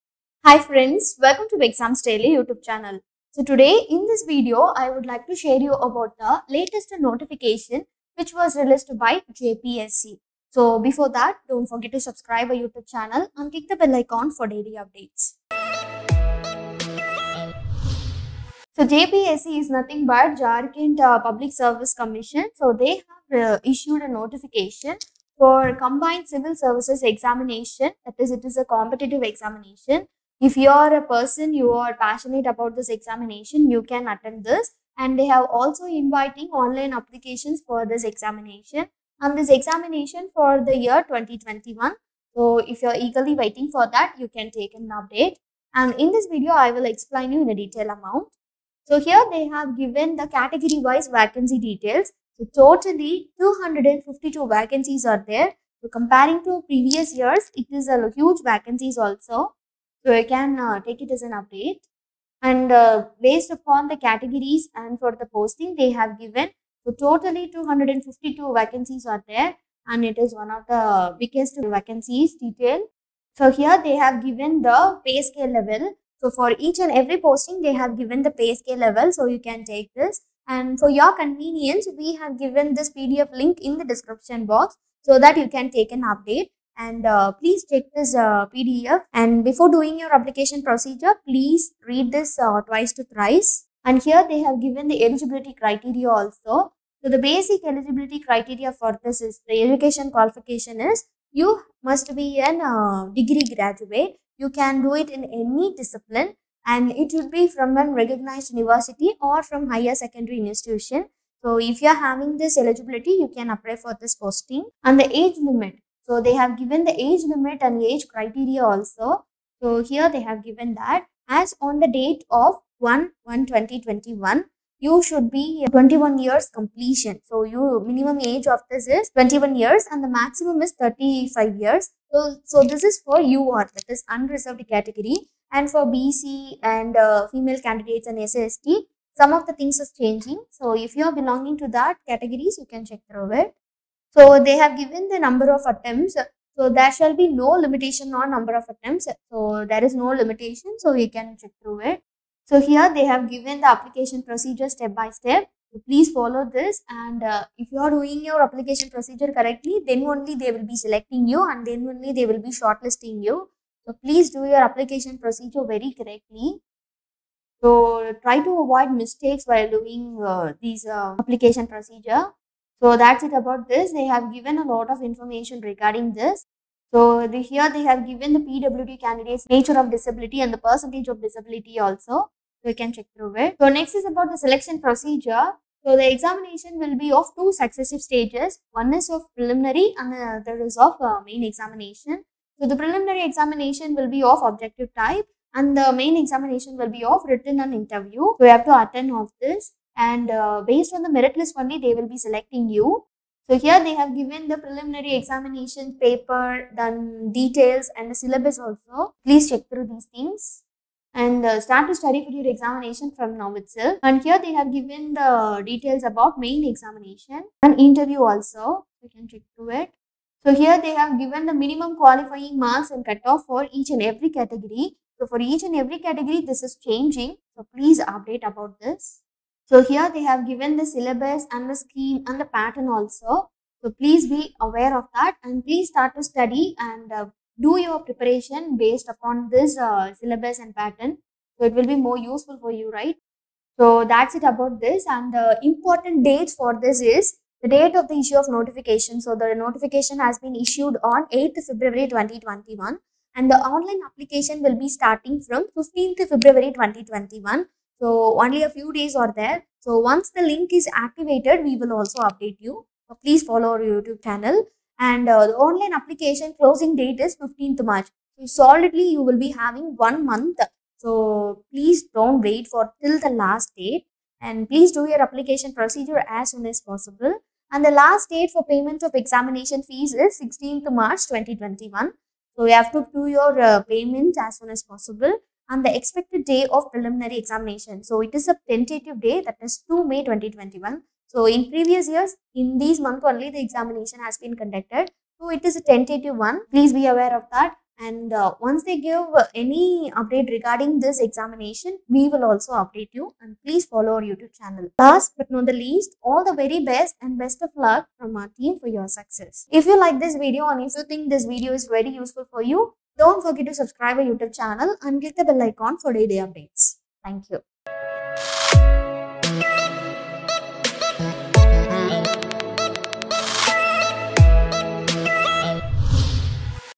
JPSC Notification 2021 – 250+ CCSE Vacancies Announced ! | Jharkhand Govt Jobs 2021
Jharkhand Public Service Commission (JPSC) has released a notification for Combined Civil Services Competition Exam 2021 in 252 Vacancies and The application process will run till 15 March 2021.